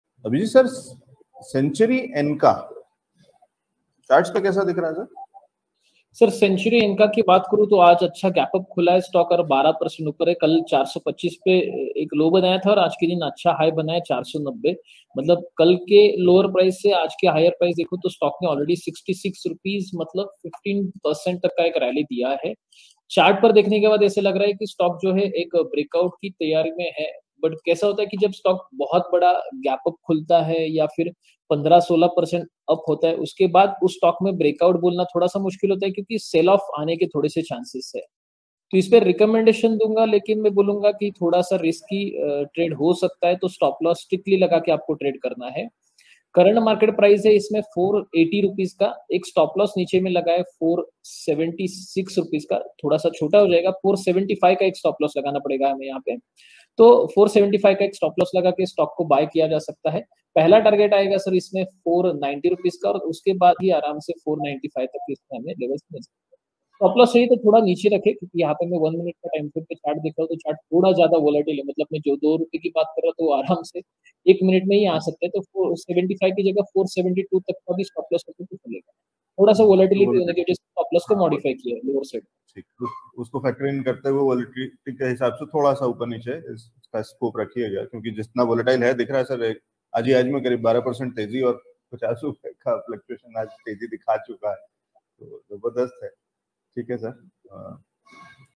Century Enka Ltd. के शेयर में क्या करें? Expert Opinion by Abhijit R Patil
जानिए हमारे Stock Market Expert Abhijit R Patil की राय |

Analysis of: Century Enka Ltd.

Shows on StockMarket equity trading investment commodity personalfinance tax cryptocurrency blockchain reviews centuryenka marketanalysis startups policyreviews nifty sensex and much more.